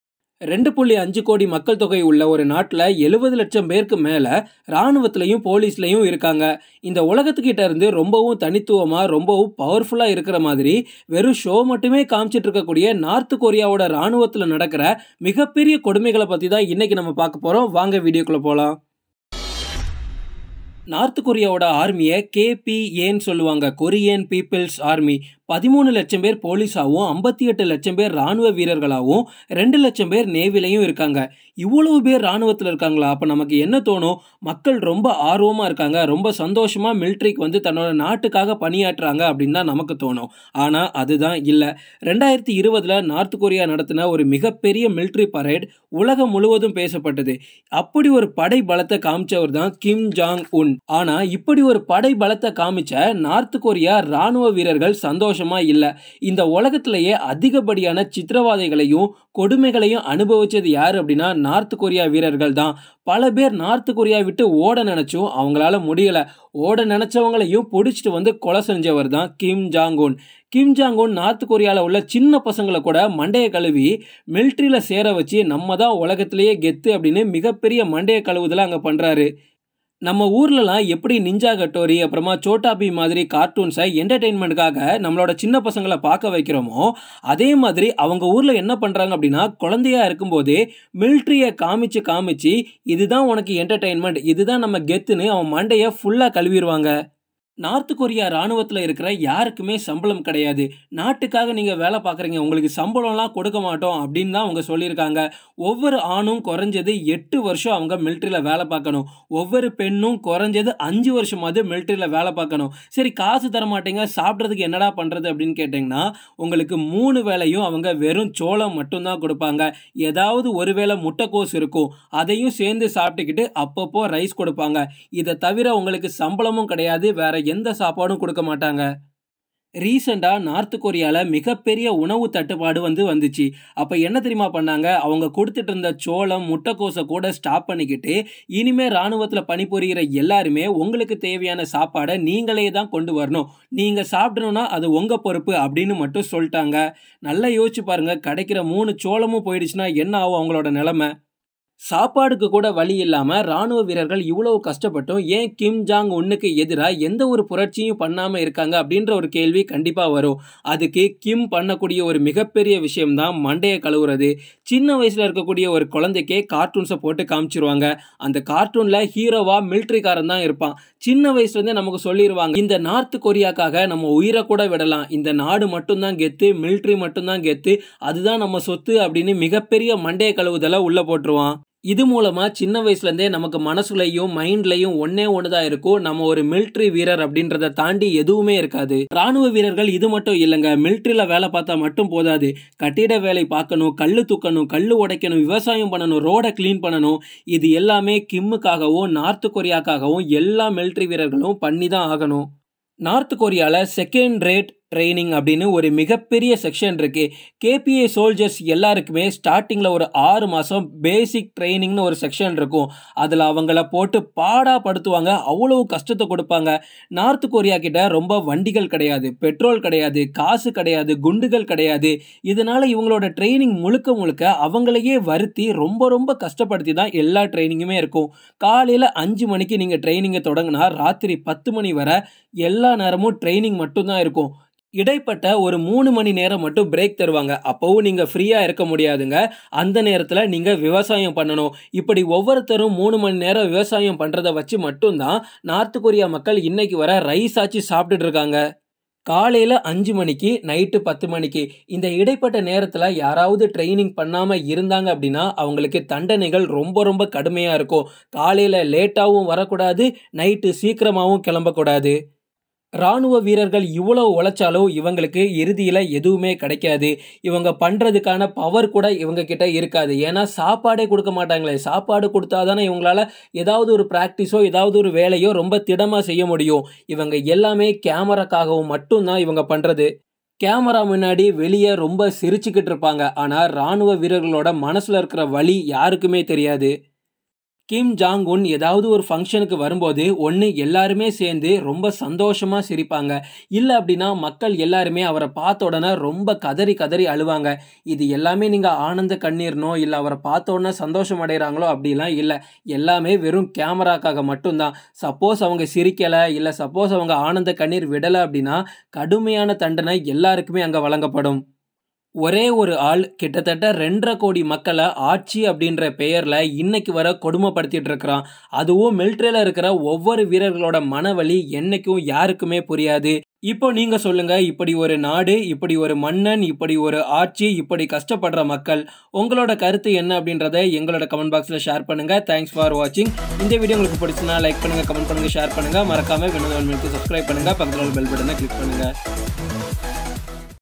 வட கொரிய ராணுவத்தில் நடக்கும் கொடுமைகள் | Horrible Things North Korean Soldiers | Vinotha Unmaigal
வட கொரிய ராணுவத்தில் நடக்கும் கொடுமைகள் | Horrible Things North Korean Soldiers Have to Go Through | Vinotha Unmaigal